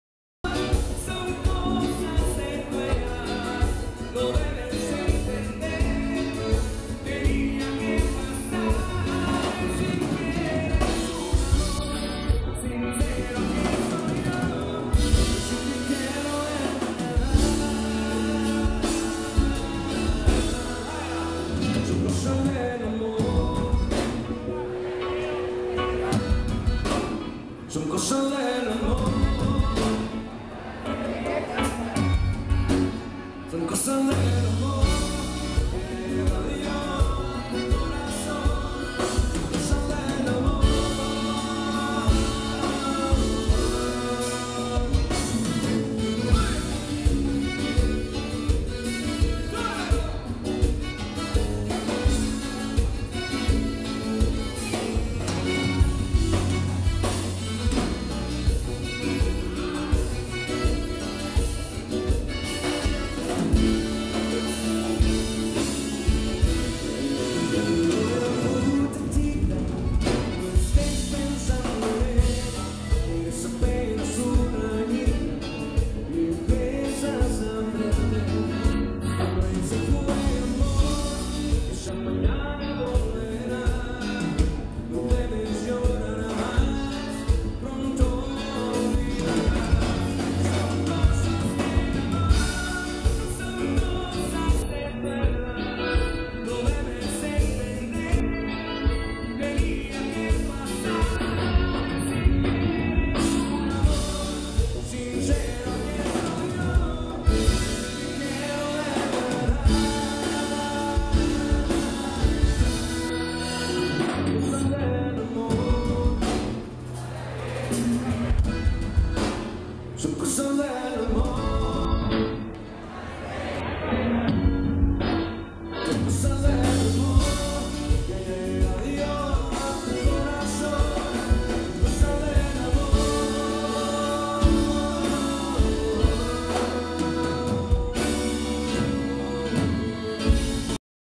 Fiesta privada 97.3 la firma
EN LA MEJOR FM CELEBRAMOS EL DIA DE REYES CON UNA GRAN ROSCA DONDE LOS INVITADOS ESPECIALES FUERON LOS CHAVOS DE LA ...

Maxibus Norteño invitado de la fiesta de la Mejor FM. 97.3, este es un mix que realizamos del evento, ojala que les agrade, esperamos sus comentarios.

en la fiesta privada de La Mejor FM 97.3.